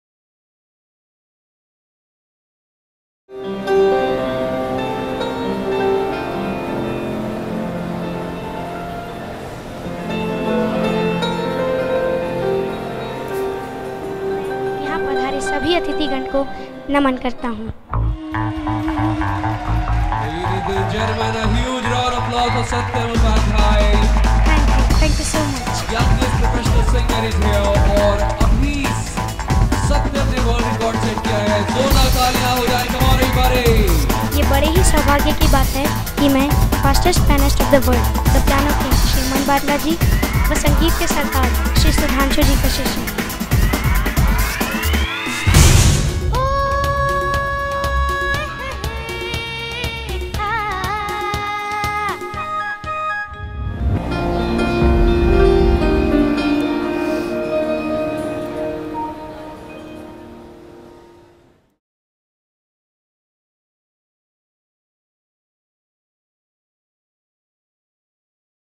SATYAM UPADHYAY || PROMO- PARWAAZ AN ASCENSIAN
Satyam Upadhyay was awarded as the Youngest Professional Singer by Golden Book of World Records on 19th February 2016 in an exclusive concert at Ambience Mall, Gurugram.